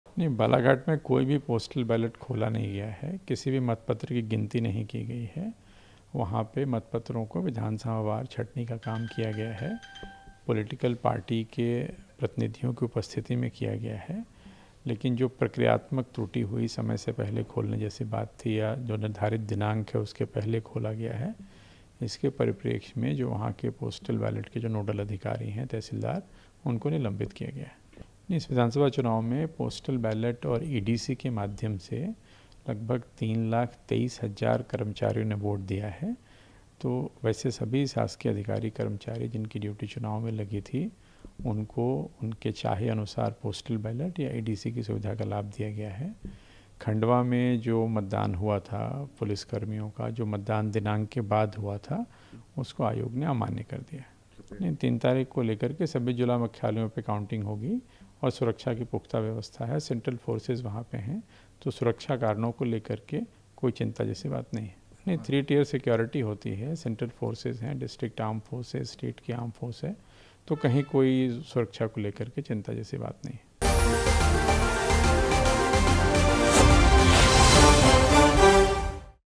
Madhya Pradesh के Balaghat में मतपेटी खुलने के Congress के आरोप पर गरमाई सियासत | Chunav Aaj Tak
मध्य प्रदेश के बालाघाट में मतगणना से पहले स्ट्रॉन्ग रूम में पोस्टल बैलेट की पेटी खुलने वाले मामले पर सियासत गरमा गई है । कलेक्टर गिरीश कुमार मिश्रा ने मुख्य निर्वाचन पदाधिकारी को इस मामले में अपनी रिपोर्ट सौंप दी है और एआरओ हिम्मत सिंह को सस्पेंड कर दिया गया है । मुख्य निर्वाचन अधिकारी अनुपम राजन ने दावा किया कि बालाघाट में कोई पोस्टल बैलट खोला नहीं गया है और किसी भी मतपत्र की गिनती नहीं की गई है । उन्होंने कहा कि वहां पर राजनीति दलों के प्रतिनिधियों की मौजूदगी में विधानसभावार छंटनी का काम किया गया है ।  निर्धारित दिनांक से पहले इस काम को करने की प्रक्रियात्मक त्रुटि की वजह से नोडल अधिकारी को निलंबित किया गया है । कांग्रेस ने मामले में बालाघाट कलेक्टर को हटाने की मांग की है । सोमवार को कांग्रेस ने एक वीडियो सोशल मीडिया पर पोस्ट किया था । साथ ही मुख्य निर्वाचन पदाधिकारी से बालाघाट में स्ट्रॉन्ग रूम खोलकर डाक मतपत्रों में हेराफेरी का आरोप लगाया था । इस मामले में कांग्रेस के प्रदेश अध्यक्ष और पूर्व मुख्यमंत्री कमलनाथ ने बालाघाट कलेक्टर के खिलाफ कार्रवाई करने की मांग की थी ।